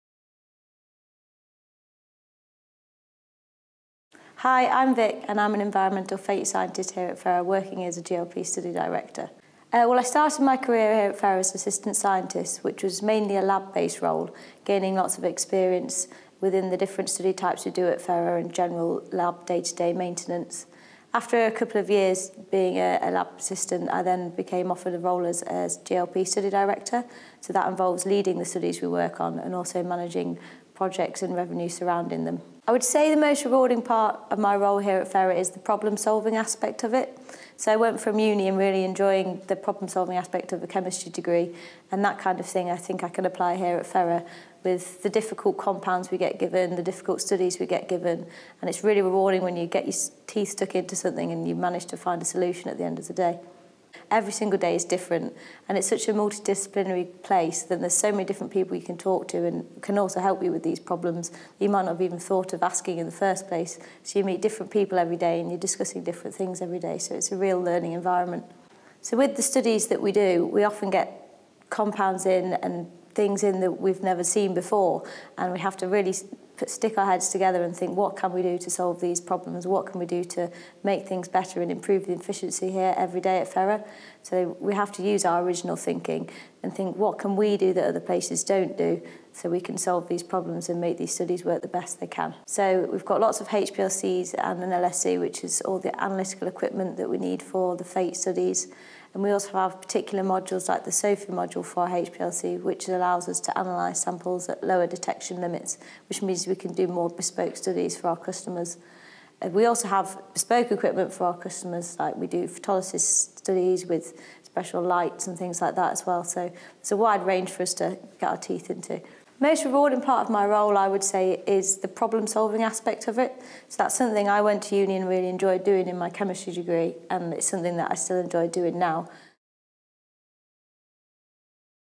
Fera Science - Victoria Peck - GLP Study Director: Environmental Fate
Join Victoria Peck discussing her role as GLP Study Director for Fera Science, across the range of studies we work on and how we use our original thinking to meet challenges every day.